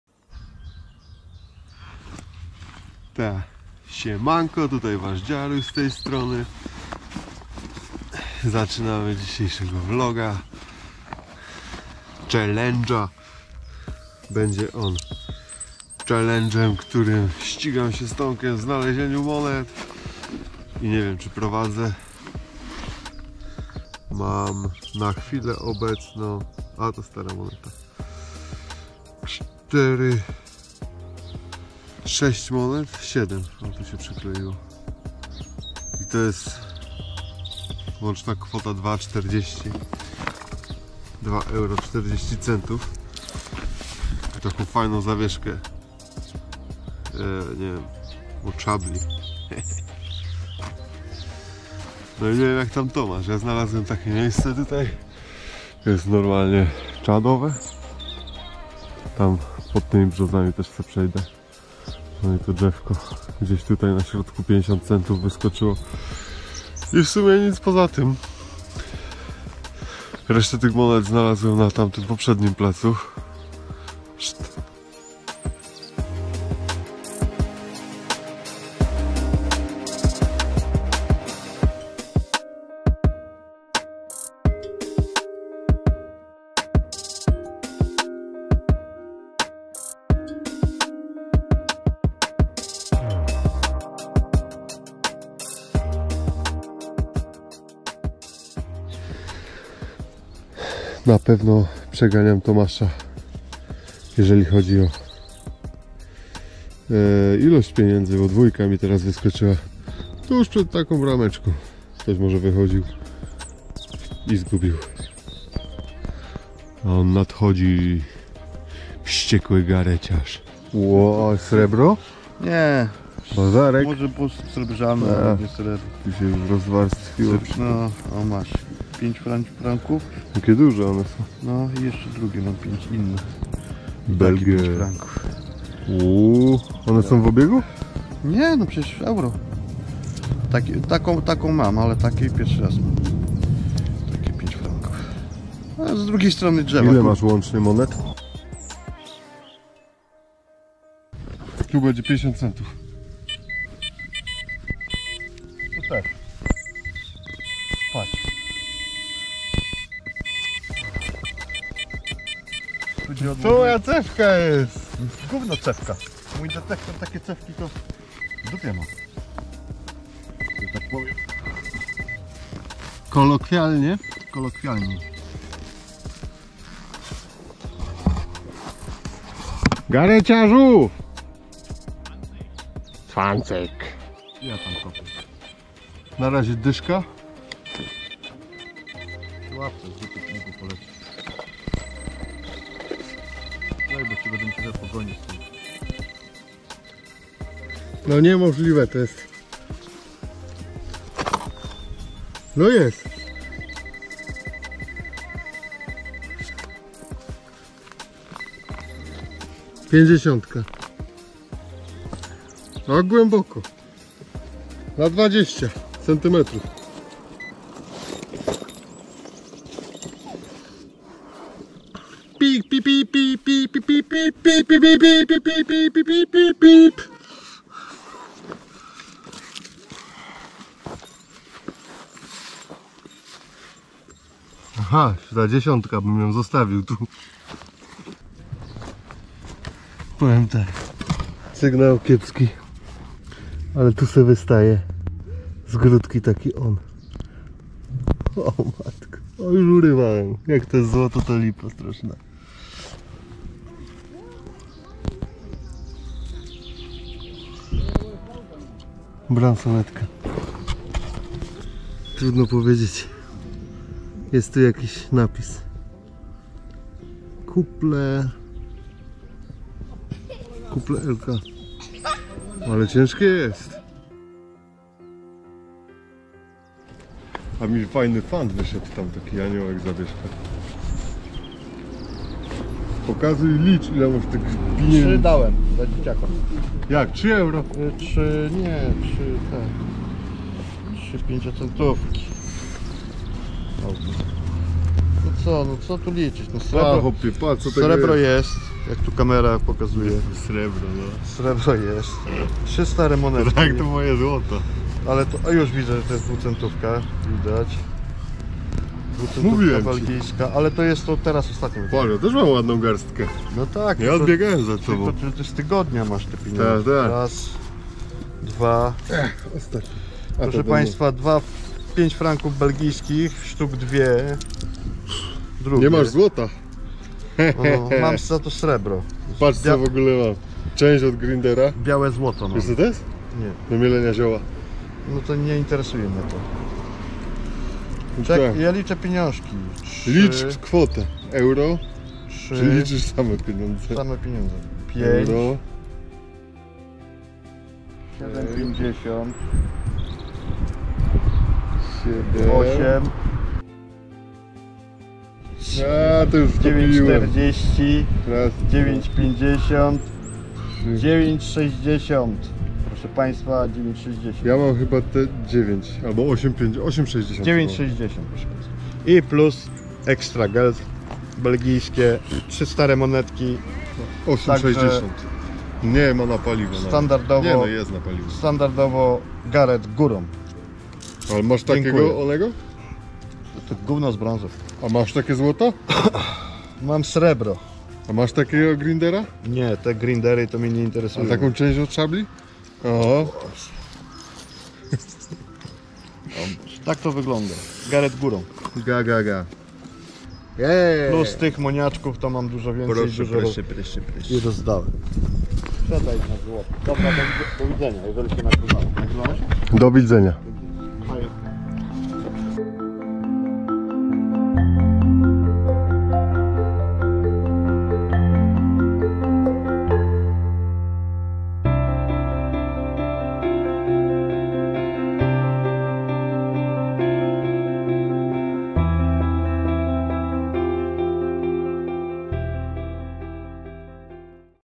Który z Nas. zarobi wiecej w 5h?
Witam szanowne grono, dzisiaj trochę inaczej. Postanowiliśmy zagrać w grę, taki czelendż, kto ma więcej ten wygrywa. Proste co? A tak sobie, wchodząc do parku musisz myśleć jak niedzielny luzak. Gdzie ułożyć kocyk, żeby nie było za daleko..? Czy to do samochodu, a może na siku w krzaki?
Moja strategia nie byłą zła, lecz nie najlepsza.
Tomek ze swoim nieposkromionym zapałem zdołał mnie przechytrzyć.
No cóż... innym razem to Ja będę lepszy.
Ściskam Dziar.